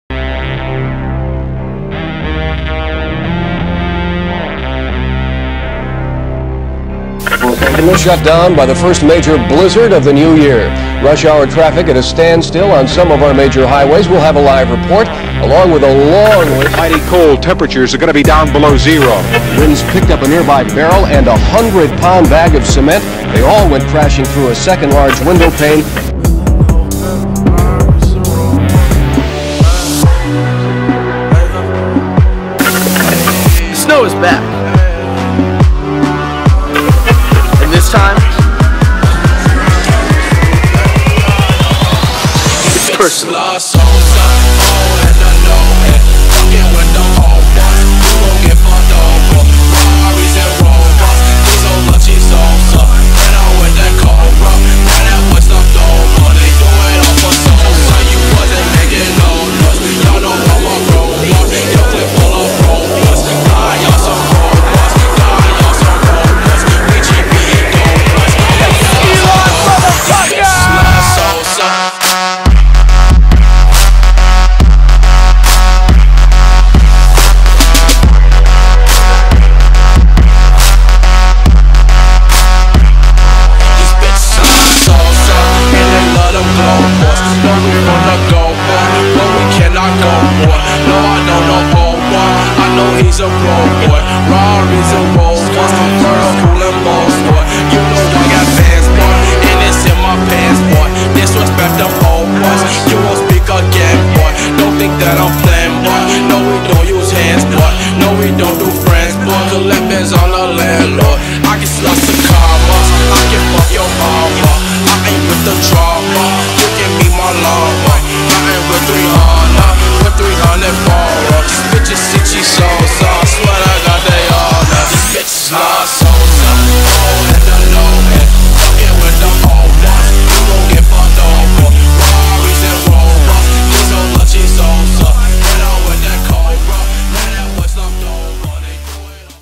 80's Ski Lodge
USC Phi Delta Theta's Fall register.
Edited by Chris Beyrooty
Party footage by Matt Prisby, Chris Beyrooty, & Hudson Leiser
Ski Footage by Jack Benziger